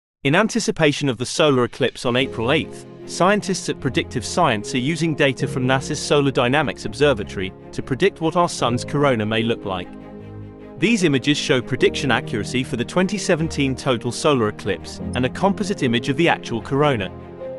What the Sun Corona May Look Like On Apr. 8
In anticipation of the solar eclipse on April 8, 2024, scientists at Predictive Science are using data from NASA’s Solar Dynamics Observatory to predict what our Sun’s corona may look like.

These images show prediction accuracy for the 2017 total solar eclipse, and a composite image of the actual corona.

Credit:
Phil Hart
Predictive Science Inc.
Miloslav Druckmüller, Tülin Bedel, Peter Aniol, Shadia Habbal